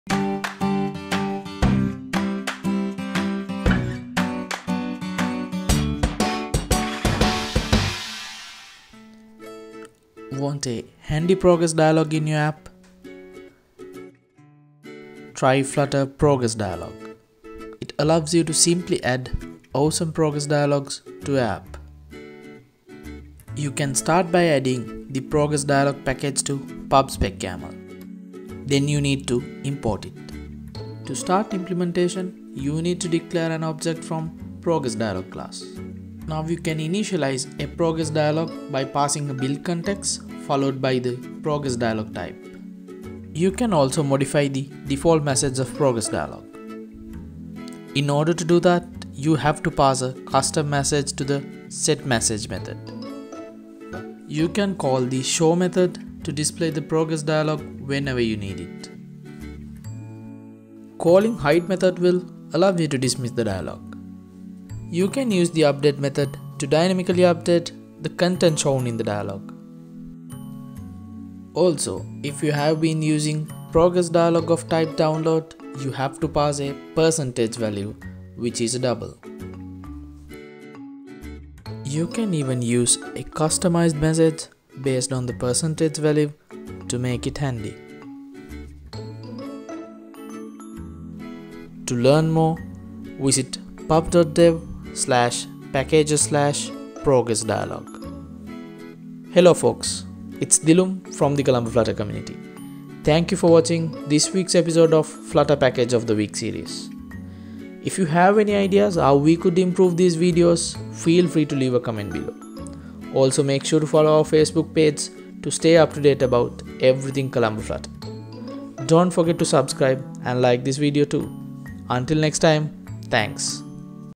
Flutter Progress Dialog (Package of the Week)
Use the Progress Dialog Package to easily create awesome progress dialogs.

Colombo Flutter Community is a Flutter community based in Colombo, Sri Lanka.